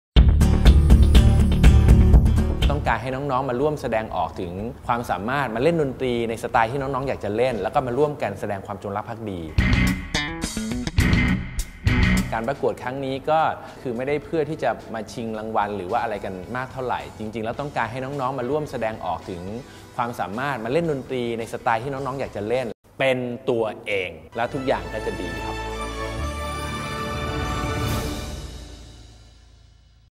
โครงการ "บทเพลงรักแห่งแผ่นดิน" ปีที่ 5 TRAINER โอ๋ ดูบาดู
โครงการ "บทเพลงรักแห่งแผ่นดิน" ปีที่ 5
เจษฎา สุขทรามร(โอ๋)

การประกวดวงดนตรีสากลร่วมสมัยระดับมัธยมศึ­­กษา ติดตามสารข่าวได้ที่